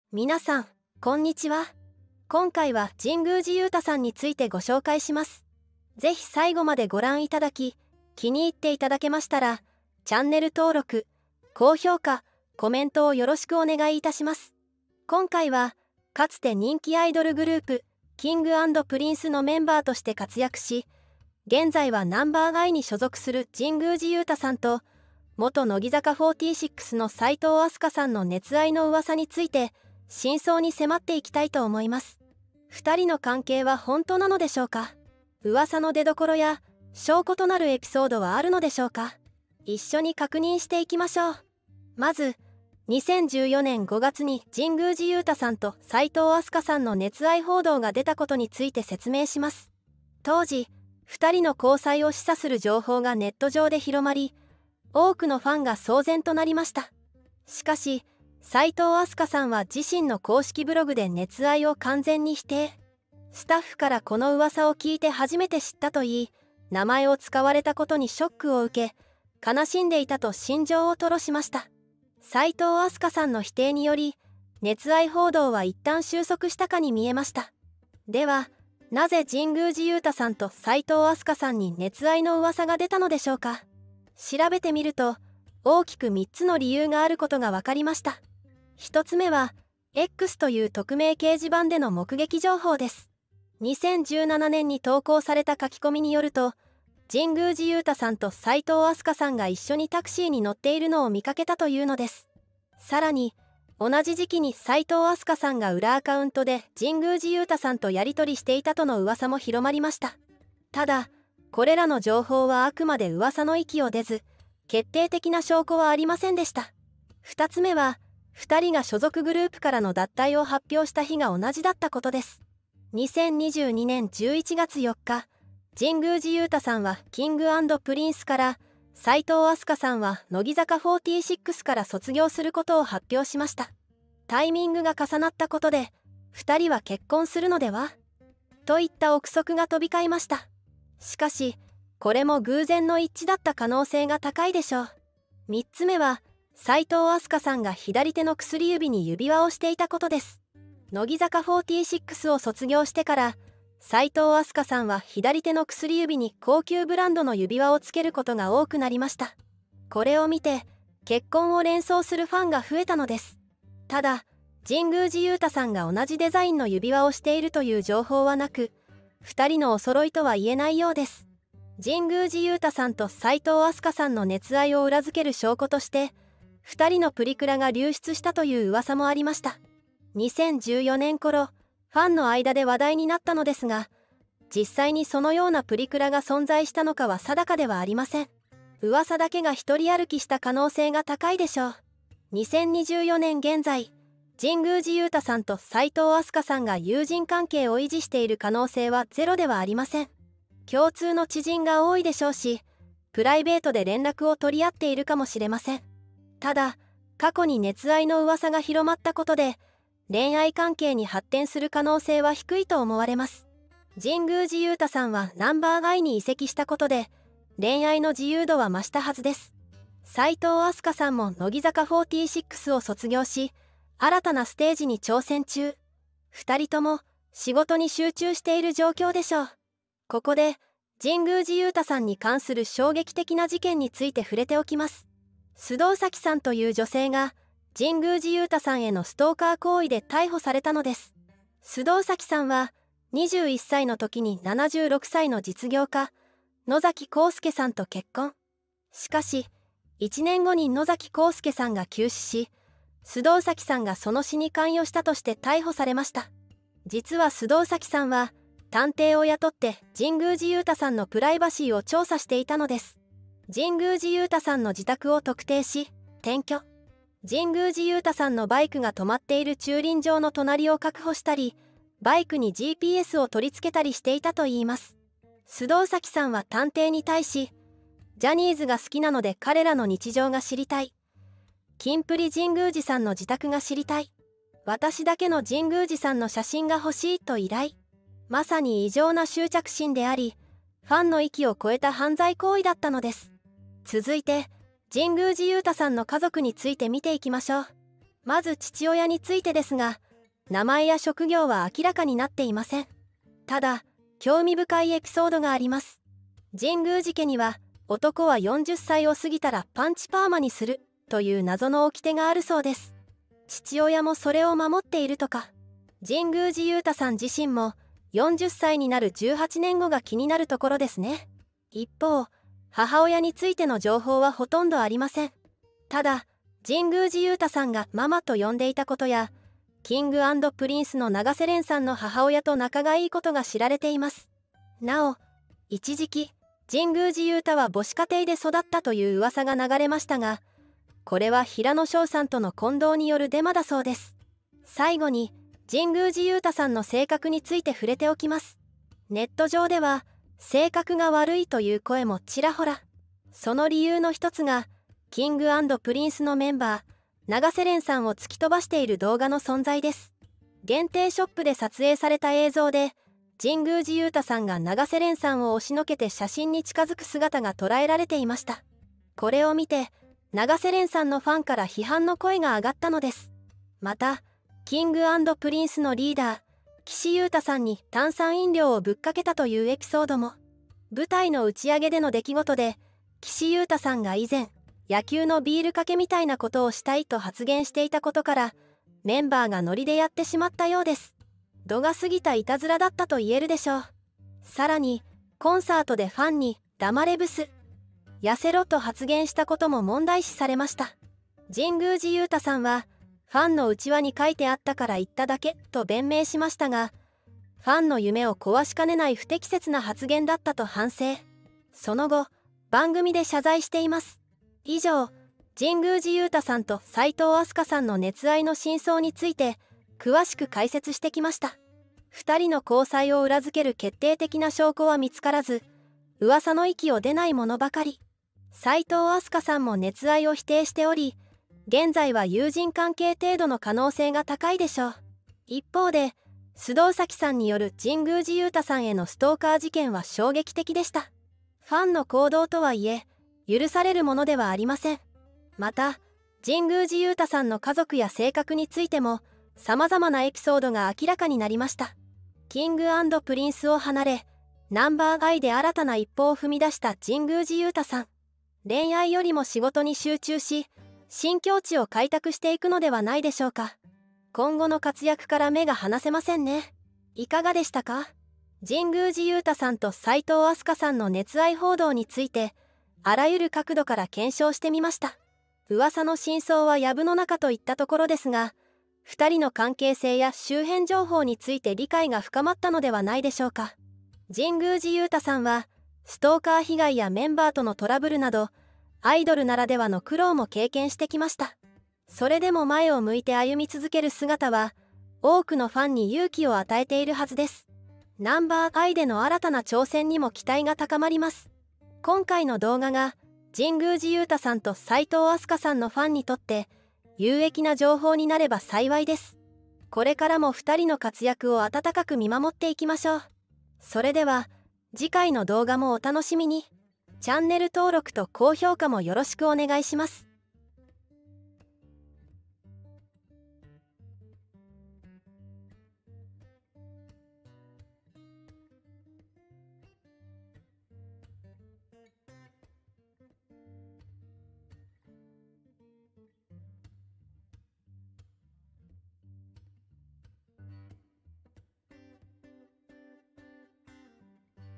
神宮寺勇太が暴露された黒い“裏の顔”…“セクシー”女優との深い関係に言葉を失う…「Number_i」として活躍するアイドルの壮絶な生い立ちに驚きを隠せない…
この動画では、アイドルグループ「Number_i」の神宮寺勇太について、

これまで知られていなかった黒い"裏の顔"が暴露されます。

実は神宮寺が、ある"セクシー"女優と極めて深い関係にあったことが明らかになります。

さらに、神宮寺の壮絶な生い立ちの実態にも切り込みます。

虐待やい●めなど、過酷な環境の中で育った経緯に、誰もが驚きを隠せないはずです。